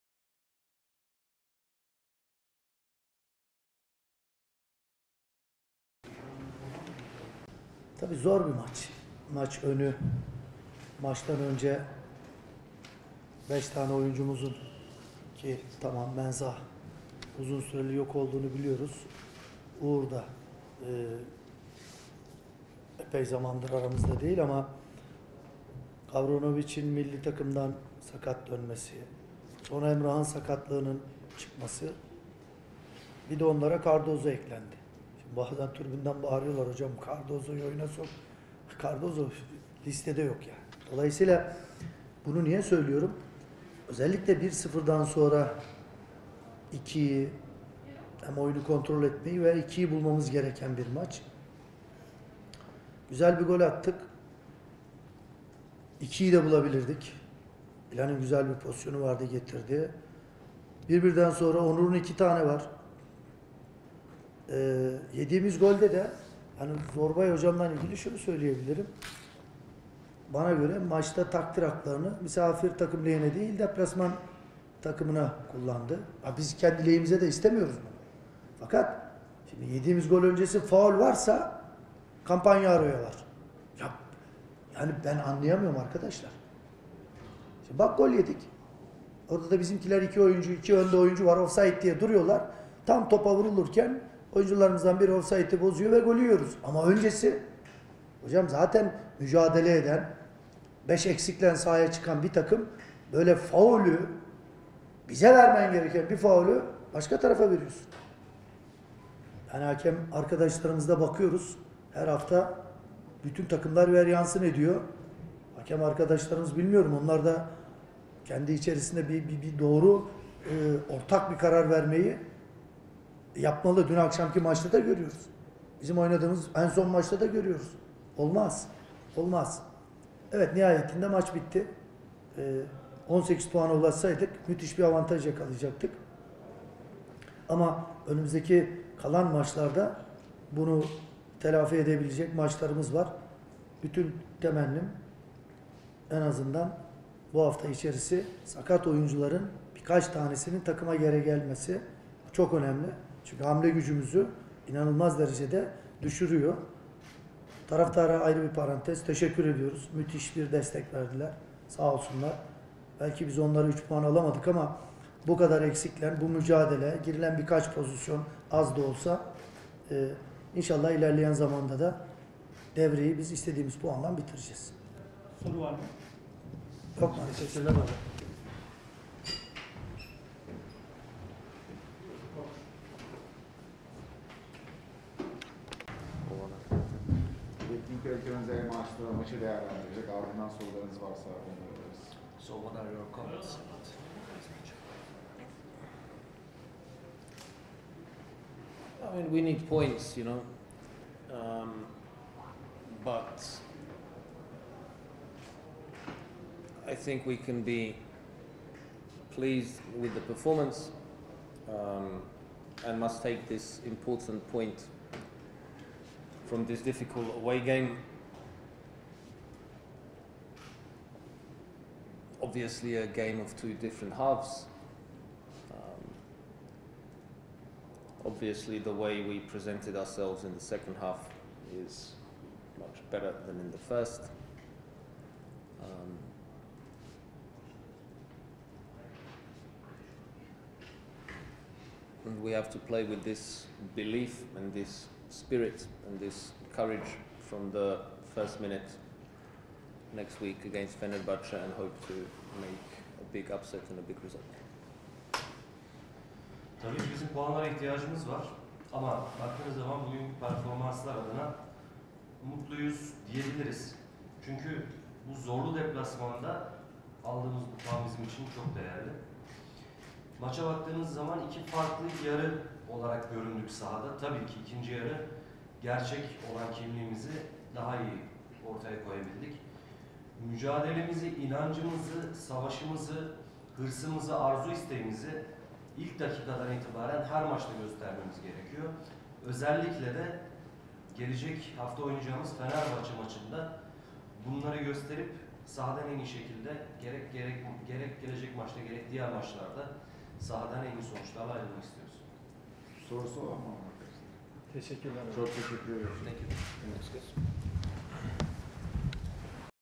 Hikmet Karaman: "Yediğimiz gol öncesi faul varsa Campanharo'ya var"
Spor Toto Süper Lig'in 13. haftasında konuk ettiği Göztepe ile 1-1 berabere kalan Yukatel Kayserispor'un teknik direktörü Hikmet Karaman, öne geçtikleri maçta sahadan bir puanla ayrıldıkları için üzgün olduklarını söyledi.

Karaman, karşılaşmanın ardından düzenlenen basın toplantısında, maç öncesi takımda 5 futbolcunun sakatlık yaşadığını ve bu mücadelede forma giyemediğini belirtti.

Hakemin tüm takdir haklarını Göztepe lehine kullandığını ileri süren Karaman, şöyle konuştu:
"Güzel bir gol attık. İkinciyi de bulabilirdik. 1-1'den sonra Onur'un iki pozisyonu var. Hakem takdir haklarını deplasman takımı adına kullandı. Biz kendi lehimize istemiyoruz. Yediğimiz gol öncesi faul varsa Campanharo'ya var. Ben anlamıyorum."
Karşılaşmadan galibiyetle ayrılacak pozisyonları bulduklarını anlatan Karaman, şunları kaydetti:
"Hakem 5 eksikle sahaya çıkan, mücadele eden takıma vermesi gereken faulü karşı tarafa veriyor. Hakemlere her hafta her takım veryansın ediyor. Onlar da kendi içerisinde bir ortak karar vermeli. 18 puana ulaşsaydık müthiş bir avantaj yakalayacaktık. Önümüzdeki maçlarda bunu telafi edebiliriz. Temennim, bu hafta içerisinde sakat oyuncularımızın birkaçının takıma dönmesi. Bu, çok önemli. Hamle gücümüzü tamamen düşürüyor. Bu kayıpları telafi edip ilerleyen zamanda da devreyi istediğimiz puanla bitireceğiz."

- Göztepe cephesi

Göztepe Teknik Direktörü Nestor El Maestro, puana ihtiyaç duyduklarını, zorlu Kayserispor deplasmanından alınan bir puanın kendileri için değerli olduğunu söyledi.

Karşılaşmanın ikinci yarısında takımının gerçek kimliğini sahaya yansıttığını anlatan Maestro, "Maçın geneline baktığımızda iki farklı takım olarak görüntü verdik. İkinci yarı gerçek kimliğimizi sahada ortaya koyabildik. Mücadelemizi, inancımızı, hırsımızı ilk dakikadan itibaren her maçta göstermemiz gerekiyor. Özellikle önümüzdeki hafta Fenerbahçe maçında göstermeliyiz." ifadelerini kullandı.